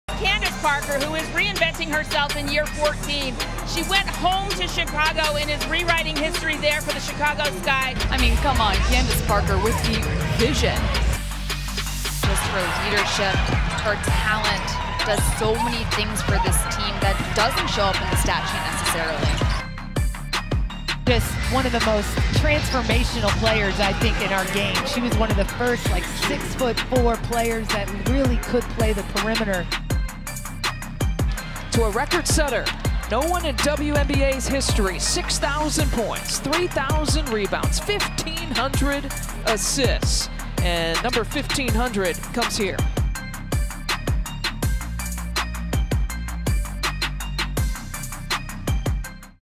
Candace Parker Best Moments With Chicago Sky, Taking Talents To Las Vegas (February 3, 2023)
A 2x WNBA Champion, Candace Parker will now set out on a new venture, signing with the reining champs the Las Vegas Aces in her 15th year for the upcoming 2023 WNBA season.